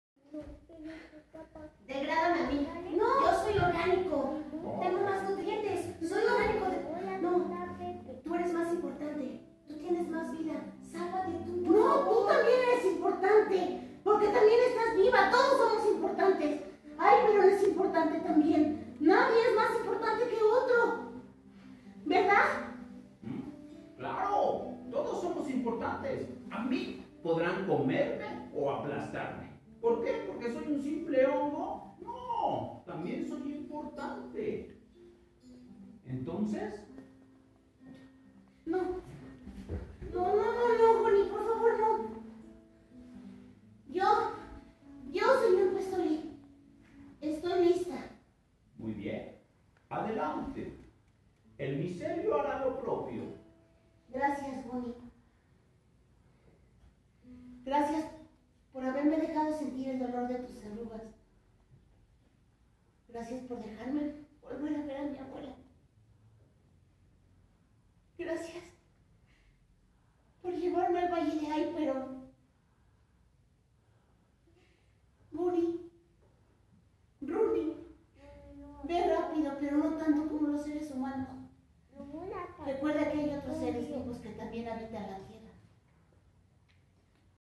Microplásticos, obra infantil. Con Lenguaje para sordos. Excelente trabajo de Ray Nolasco.
Microplásticos, la obra. Excelente trabajo de Ray Nolasco y Adriana Reséndiz  octubre 2021 Con lenguaje para sordos. Noticieros Teatrikando.